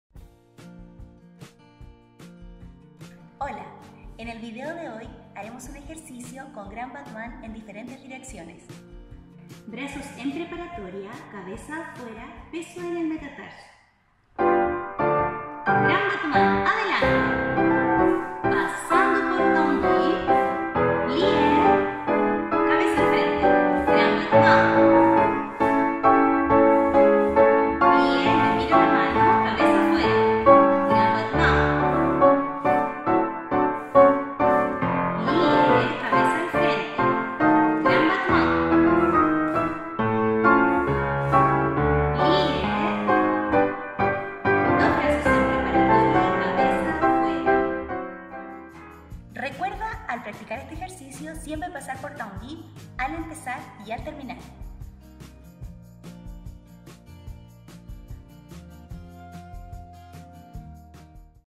Clase 53. Ejercicio Grand Battement
¡Hola bailarinas y amantes del ballet! Aquí les dejamos un ejercicio para que apliquen sus conocimientos aprendidos en los vídeos anteriores. ¡Sigamos aprendiendo juntas!